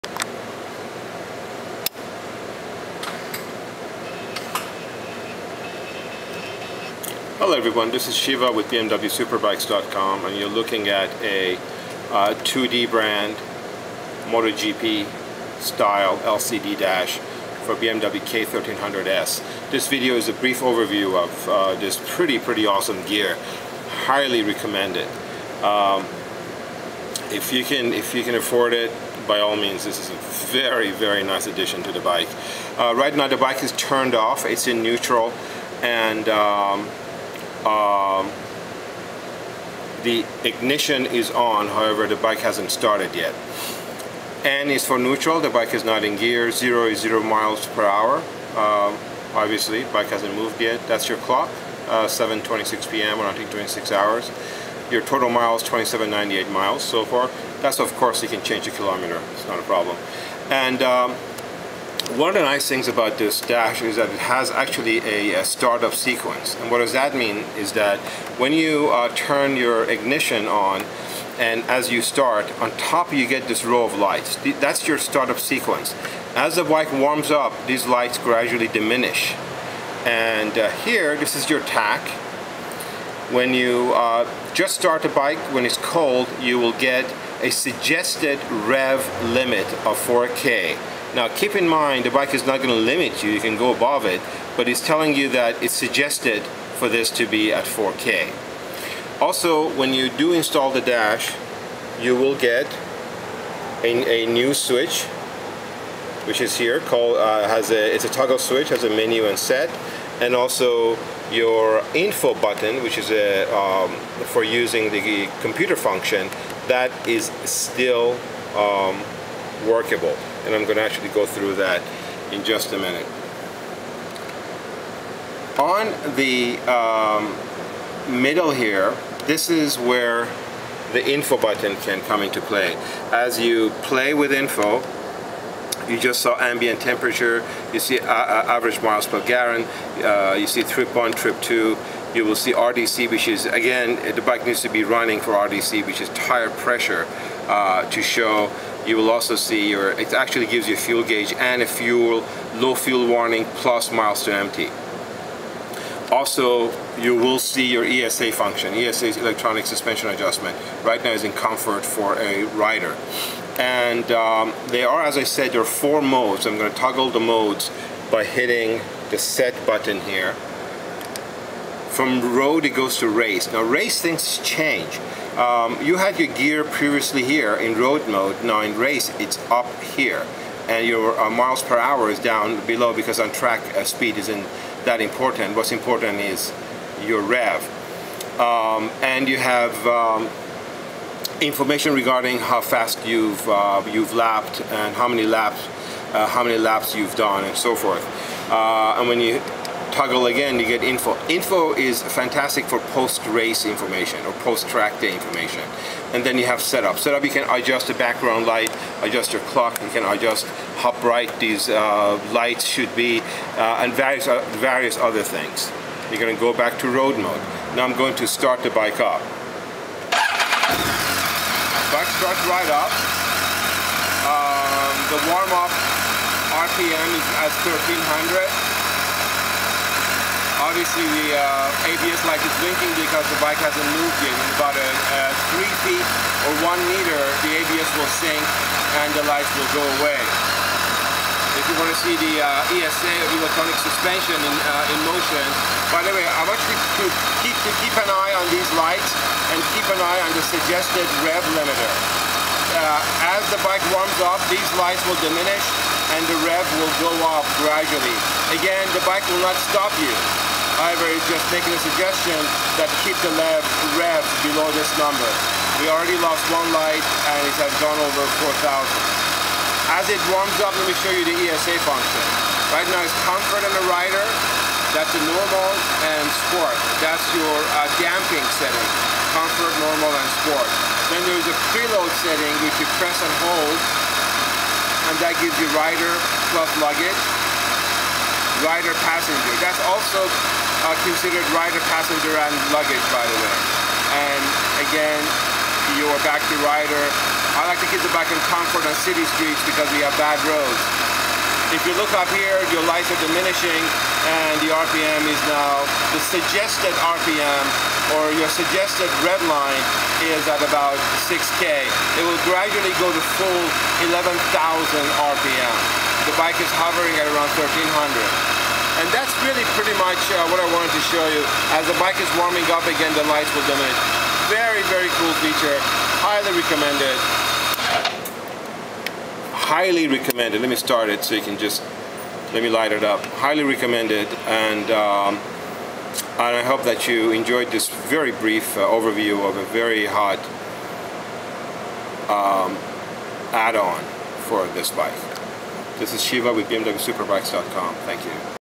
2D MotoGP LCD Dash on BMW K1300S - Overview of 4 Modes + Features
2D MotoGP LCD Dash on BMW K1300S - Overview of 4 Modes plus Features, including ESA, Tire Pressure, Start-up sequence and more.  The wheels need to sync in the first 1 meter of riding.